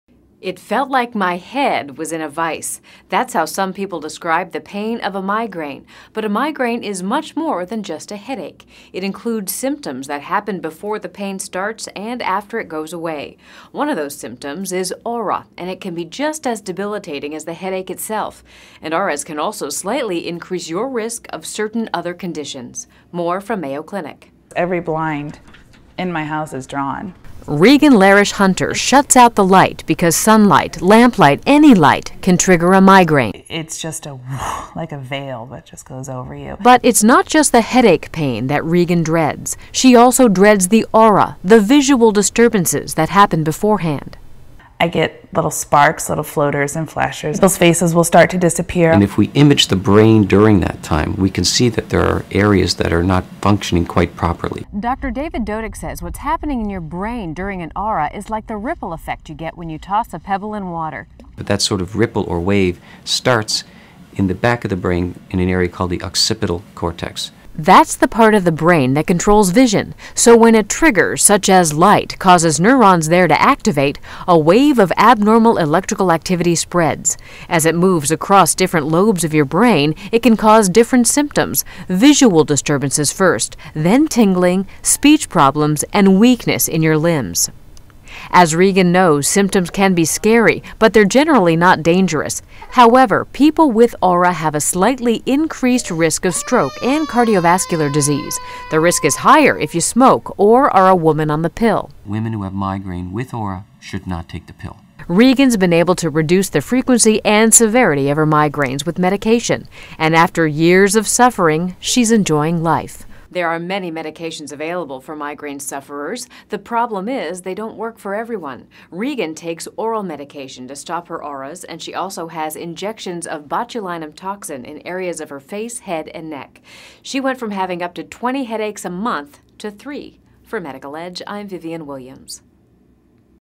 Migraine and Aura-Mayo Clinic
It felt like my head was in a vice. That's how some people describe the pain of a migraine. But a migraine is much more than just a headache. It includes symptoms that happen before the pain starts and after it goes away. One of those symptoms is aura, and it can be just as debilitating as the headache itself. And auras can also slightly increase your risk of certain other conditions. More from Mayo Clinic.